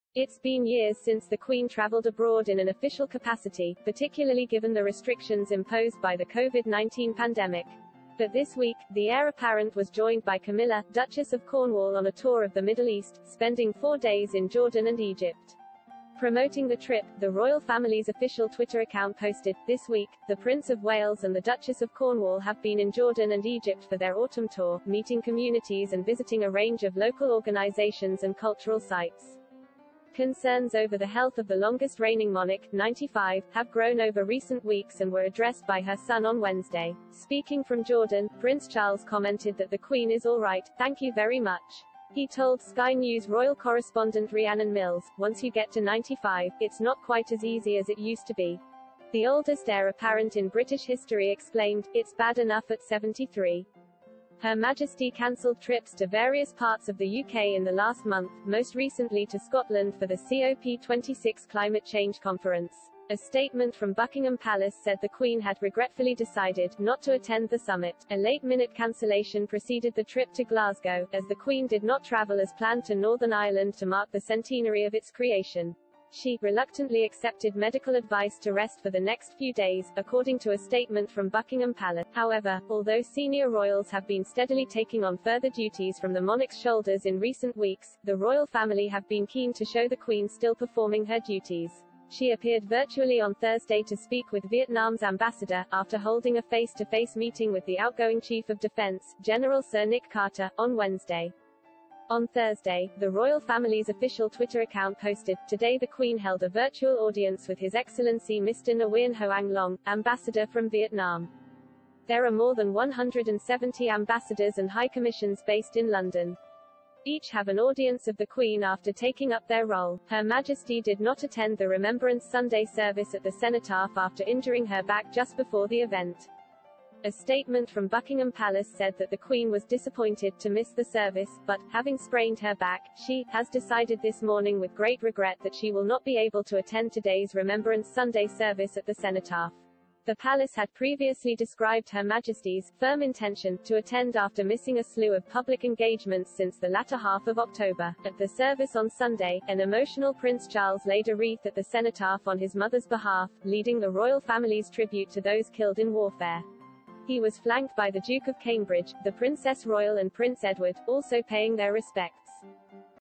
QUEEN Elizabeth II is coming to depend on her son, Prince Charles, to take her place in royal engage
QUEEN Elizabeth II is coming to depend on her son, Prince Charles, to take her place in royal engagements more and more as the monarch advances in age.
Prince Harry and Meghan Markle,
Prince Harry,
Meghan Markle,